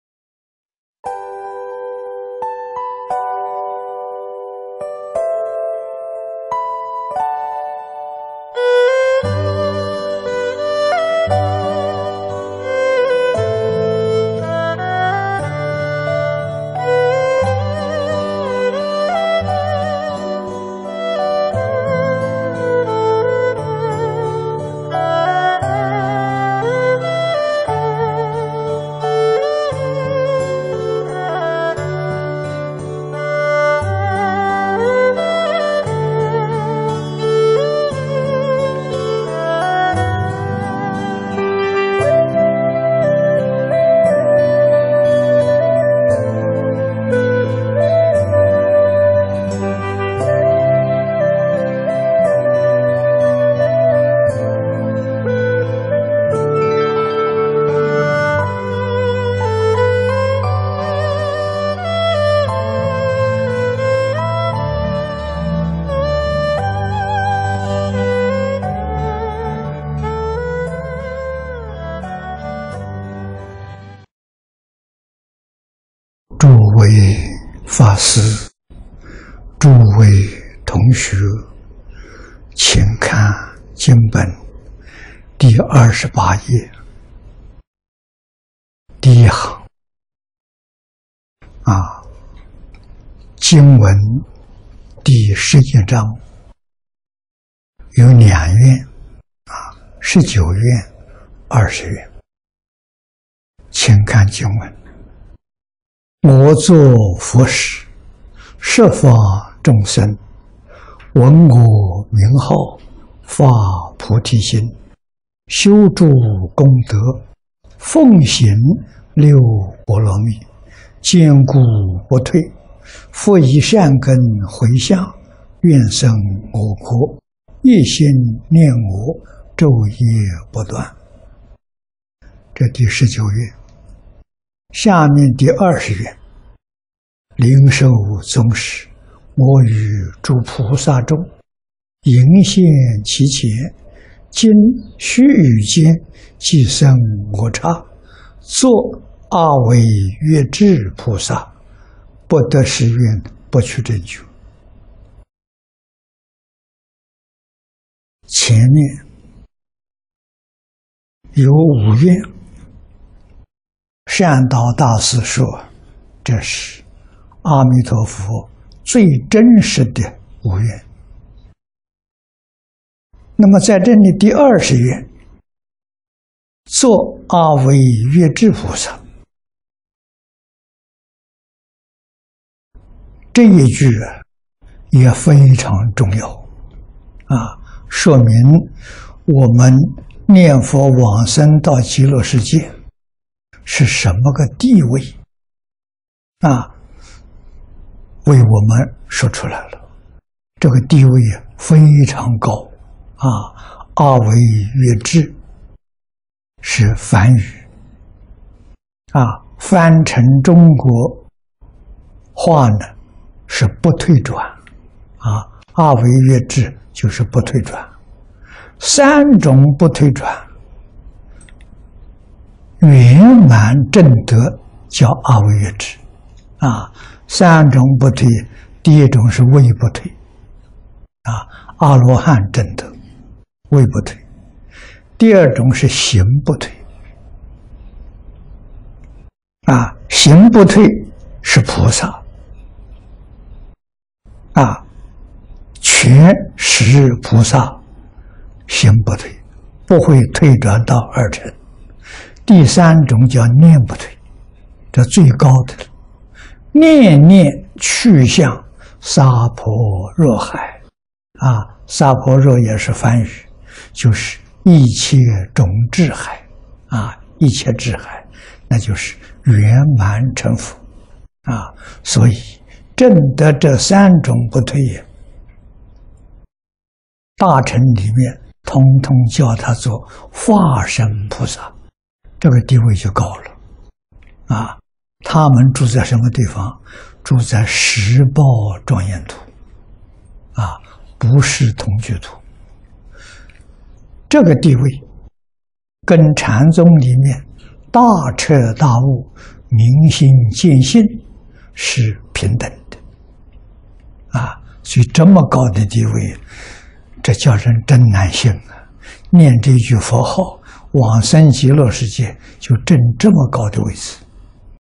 淨空老法師:發大誓願（第二回）（第六集）01/24
節錄自~檔名：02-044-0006，日本四季之湯溫泉遺產酒店

影片字幕：
諸位法師，諸位同學，請看經本第二十八頁第一行，經文第十一章有兩願，十九願、二十願。請看經文：
       【我作佛時。十方眾生。聞我名號。發菩提心。修諸功德。奉行六波羅蜜。堅固不退。復以善根迴向。願生我國。一心念我。晝夜不斷。】
       這是第十九願。下面第二十願：
       【臨壽終時。我與諸菩薩眾迎現其前。經須臾間。即生我剎。作阿惟越致菩薩。不得是願。不取正覺。】
       前面有五願，善導大師說這是阿彌陀佛最真實的五願。在這裡第二十願，『作阿惟越致菩薩』，這一句也非常重要，說明我們念佛往生到極樂世界是什麼個地位，為我們說出來了，這個地位非常高。阿惟越致是梵語，翻成中國話是不退轉，阿惟越致就是不退轉，三種不退轉圓滿證得叫阿惟越致。三種不退，第一種是位不退，阿羅漢證得位不退；第二種是行不退，行不退是菩薩，權實菩薩行不退，不會退轉到二乘；第三種叫念不退，這最高的了，念念趣向薩婆若海。薩婆若也是梵語，就是一切種智海，一切智海就是圓滿成佛。所以證得這三種不退，大乘裡面統統叫他做法身菩薩，這個地位就高了，他們住在什麼地方？住在實報莊嚴土，不是同居土。這個地位跟禪宗裡面大徹大悟、明心見性是平等的。所以這麼高的地位這叫人真難信，念這句佛號往生極樂世界就證這麼高的位次。